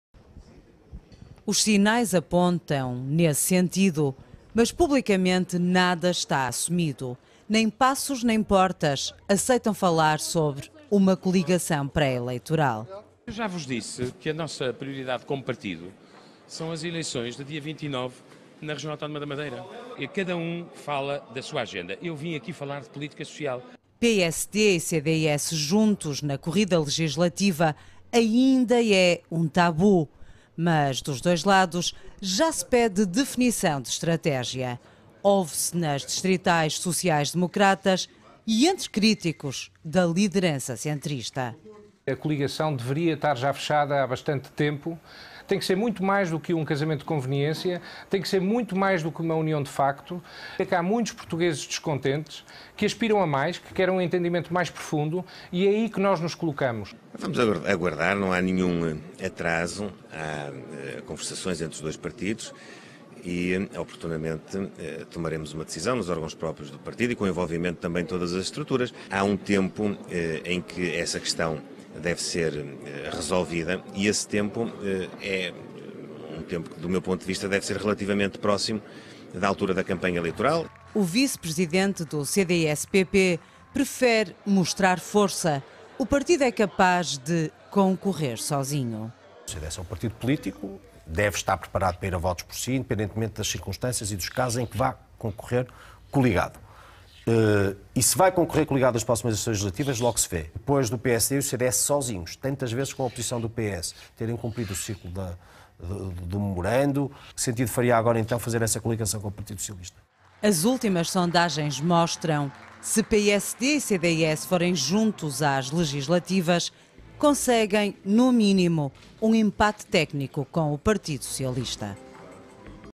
Vozes de PSD e CDS PP pedem definição de estratégia
Há cada vez mais figuras do PSD e do CDS-PP que pedem um esclarecimento sobre a coligação para as próximas legislativas. Paulo Portas adia a questão. Anacoreta Correia esperava mais celeridade. .